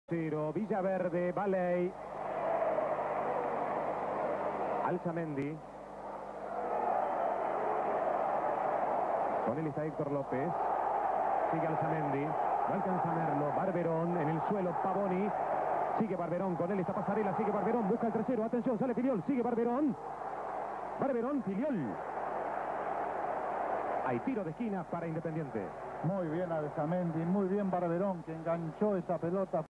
BALEY- ALZAMENDI- BARBERÓN: arranca la contra, llena de lujos. Final Nacional 1978
Independiente Bicampeón Nacional. Ricardo Bochini, goleador, genio y figura…una vez más.
Torneo Nacional 1978- La Final. Partido de Vuelta
Jugado el 10/01/1979 en La Doble Visera de Avellaneda, el Rojo sale campeón al derrotar 2-0 a River con dos del Maestro.
El equipo del PATO: Independiente: Héctor Baley; Rubén Pagnanini (Horacio Insaurralde), Hugo Villaverde, Enzo Trossero y Osvaldo Pérez; Omar Larrosa, Carlos Fren y Ricardo Bochini; Antonio Alzamendi (Fontana), Norberto Outes y Alejandro Barberón. DT: José O. Pastoriza.
River Plate: Ubaldo Fillol; Eduardo Saporiti, José L. Pavoni, Daniel Passarella y Héctor López; Juan J. López, Reinaldo Merlo y Norberto Alonso; Pedro González (Rubén Galletti), Leopoldo Luque y Oscar Ortiz. DT: Angel Labruna.